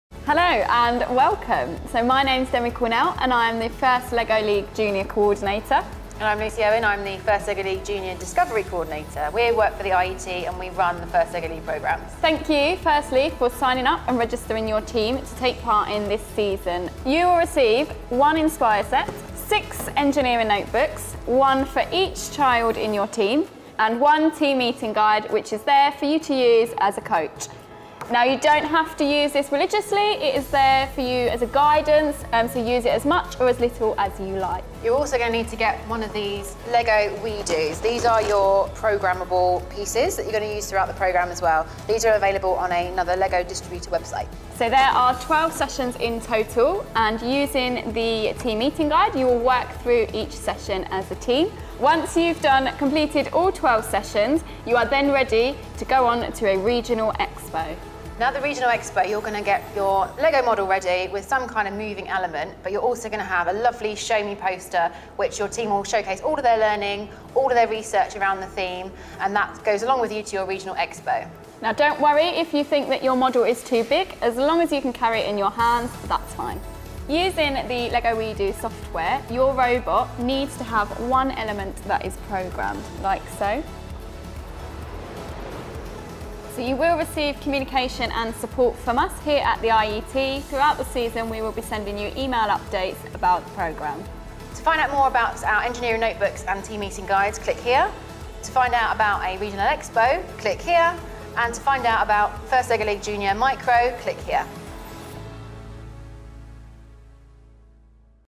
Welcome to the IET FIRST® LEGO® League Jr. 2019-20 season!
Demi and Lucy from the IET FIRST® LEGO® League team welcome you to the BOOMTOWN BUILD season! Find out what to expect when you register, what kit you need, and where to find all the information to complete this season’s awesome challenge!